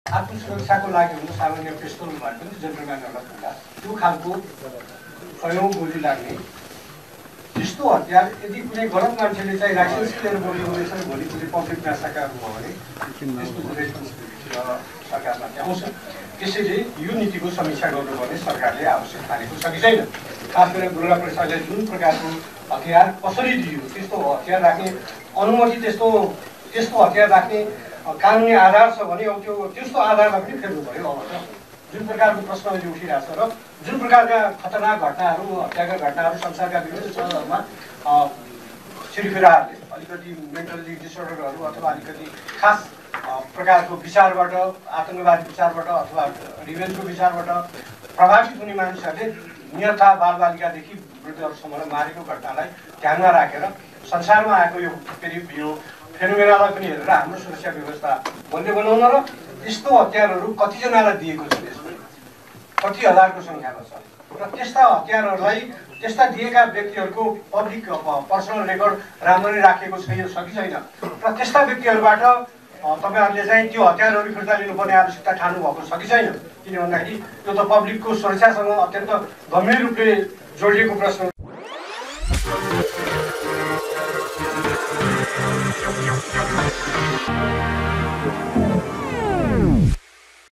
'दुर्गा प्रसाईँका अङ्गरक्षकलाई अनुमति नै नभएको हतियार राख्न कसरी दिइयो ?' : Raghuji Panta
'दुर्गा प्रसाईँका अङ्गरक्षकलाई अनुमति नै नभएको हतियार  राख्न कसरी दिइयो ?'